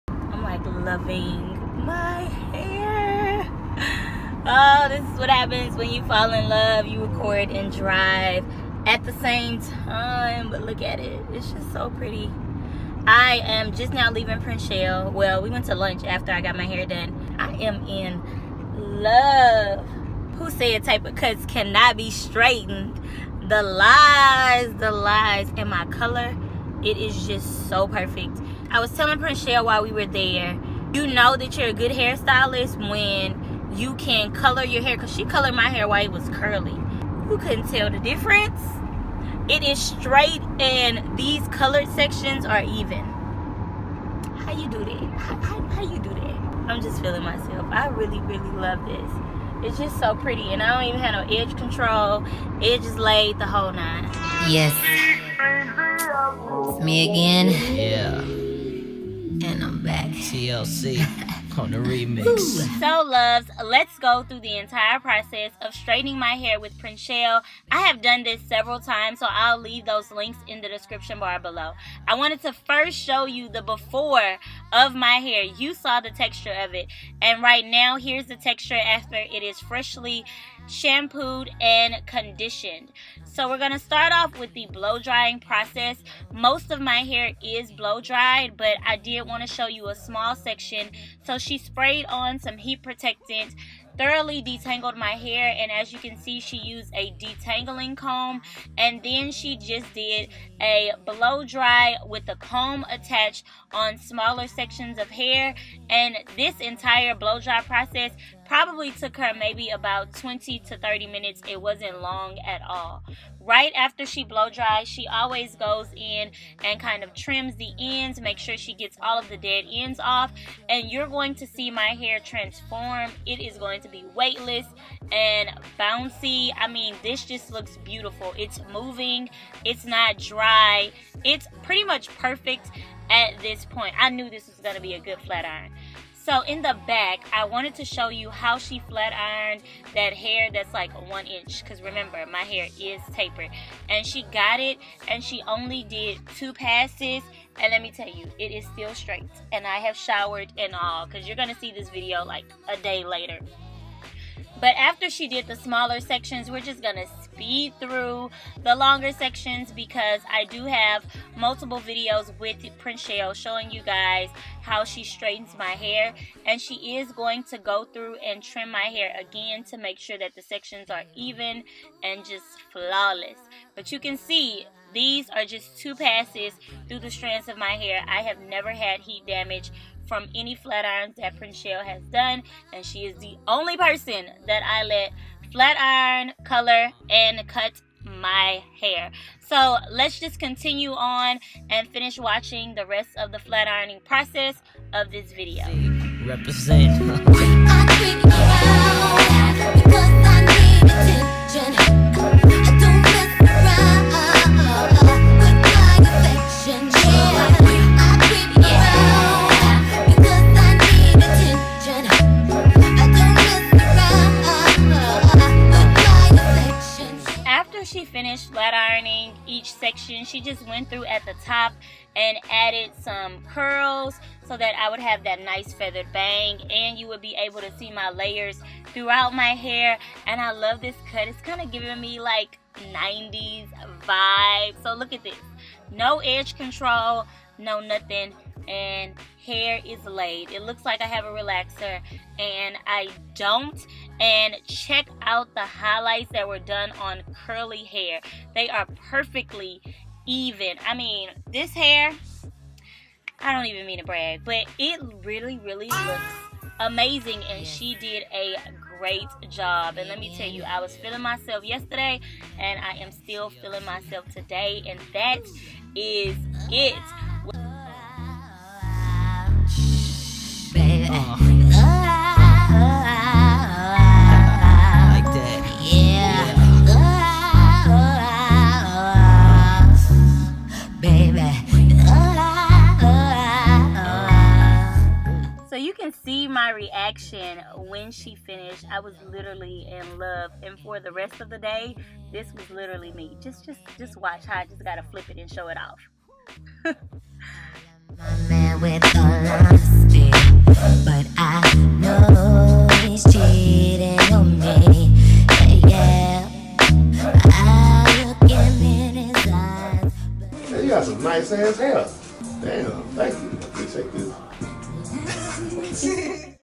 My Natural Hair Tapered Cut is STRAIGHT!!!
I got my natural hair tapered cut straightened for the first time and I documented the entire experience in this video.

Prinshell Williams
Hairbyprinshell

[[[ get FREE products]]]

E D I T I N G & C A M E R A
Canon 70D